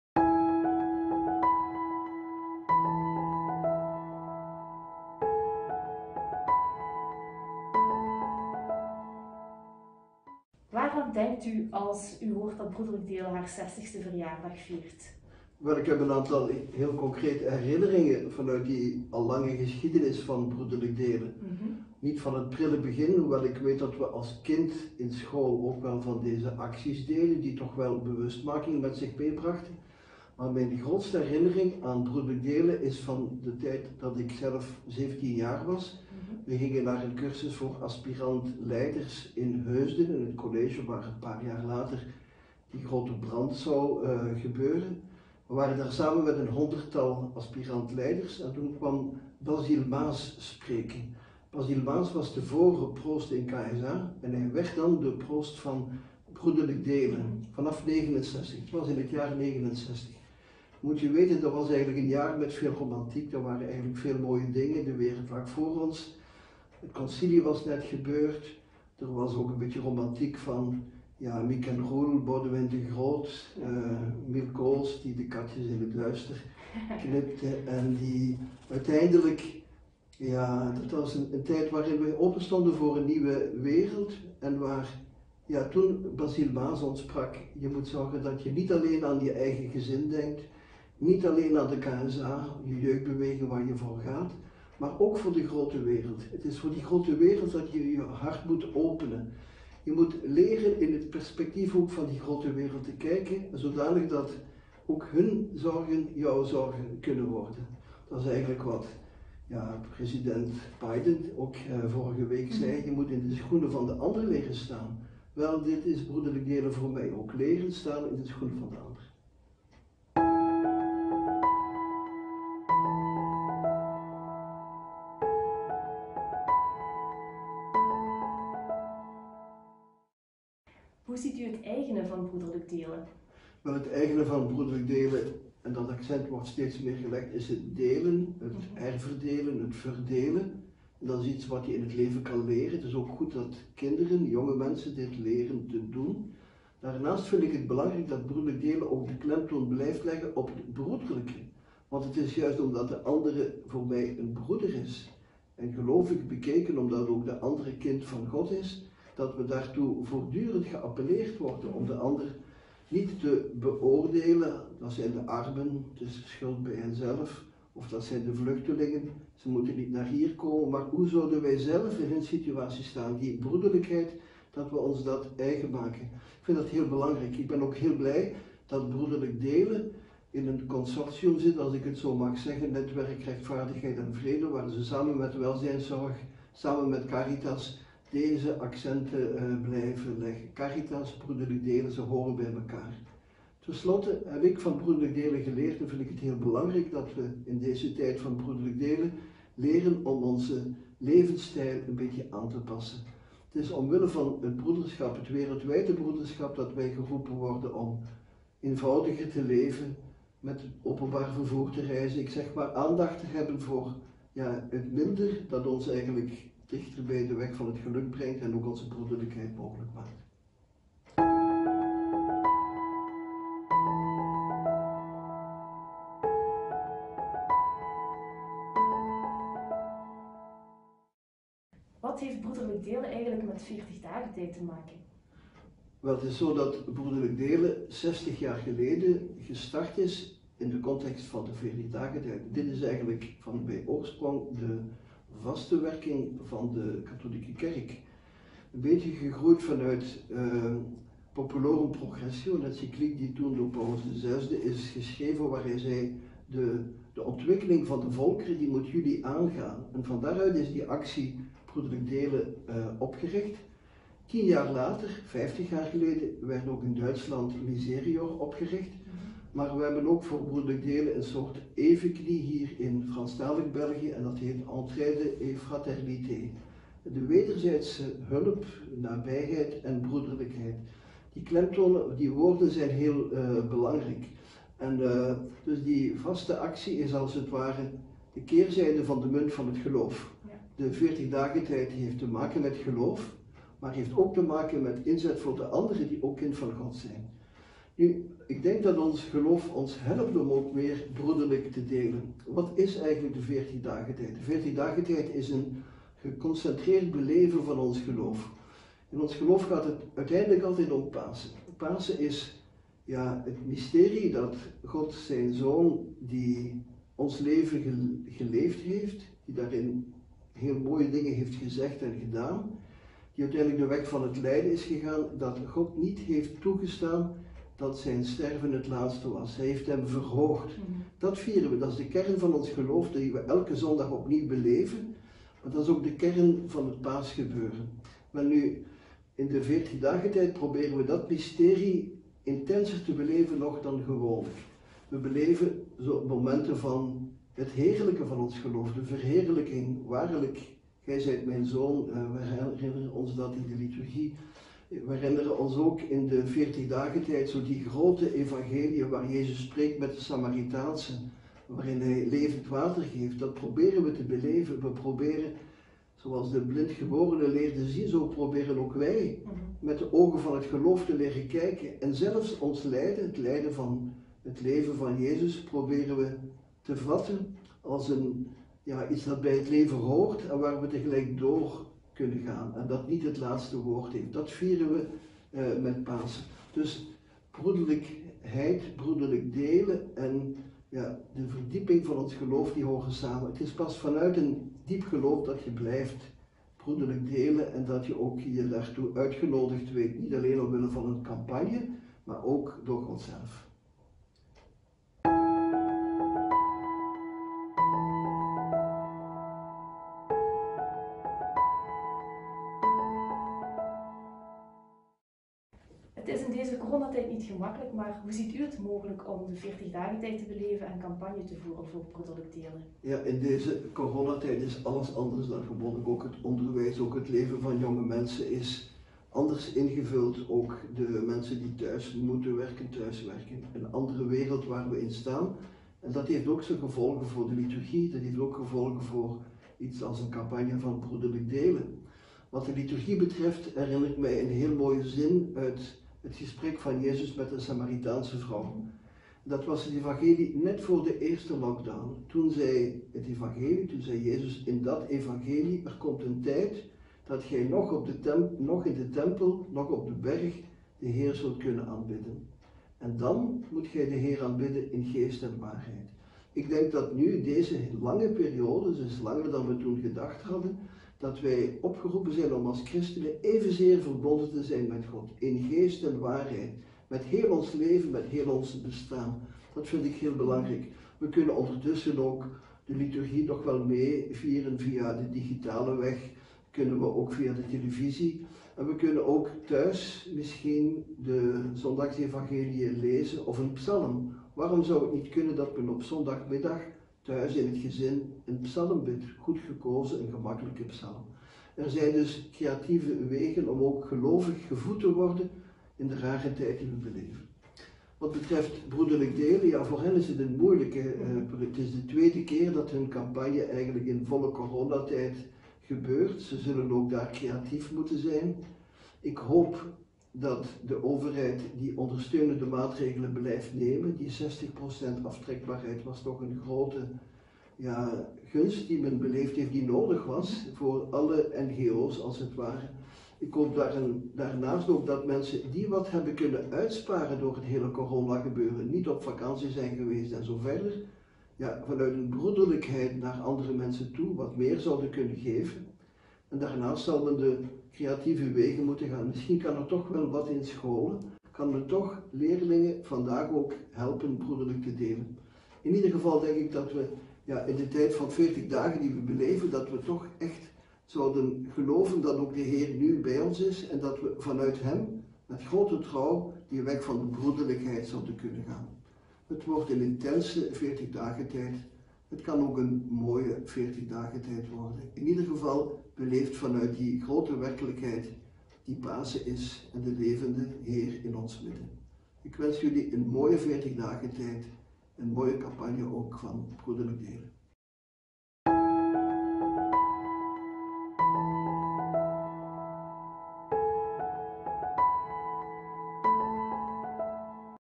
Videboodschap Mgr Hoogmartens bij de start van de veertigdagentijd
In deze video staat mgr. Patrick Hoogmartens even stil bij 60 jaar Broederlijk Delen en het eigene van Broederlijk Delen. Hij belicht verder de link tussen Broederlijk Delen en de veertigdagentijd. Tot slot spreekt hij over veertigdagentijd en campagne voeren in coronatijd.